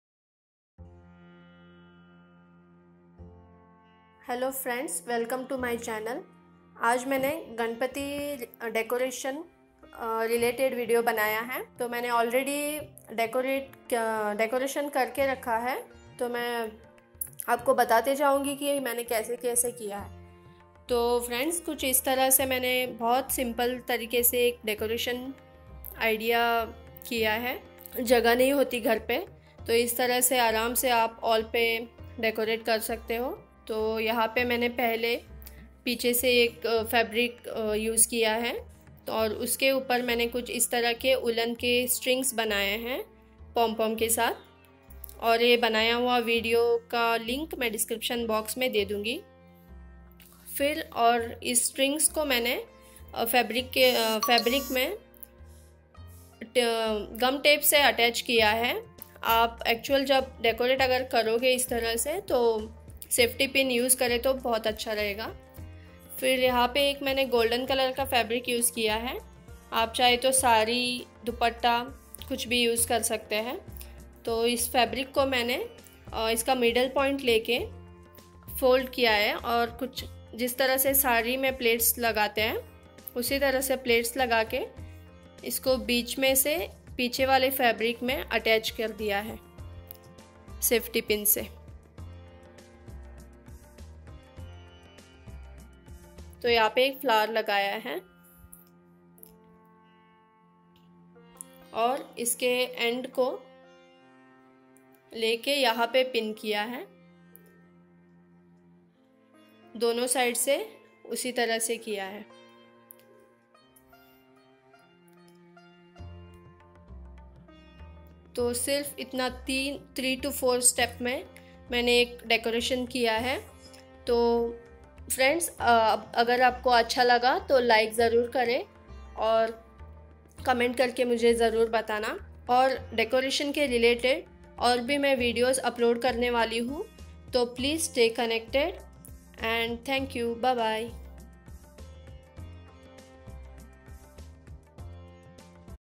Ganapati decoration
With Ganesh Chaturthi approaching soon, I thought it would be nice to share some ideas on how we could decorate a place for Lord Ganapati at home.

Name: Multimedia Production World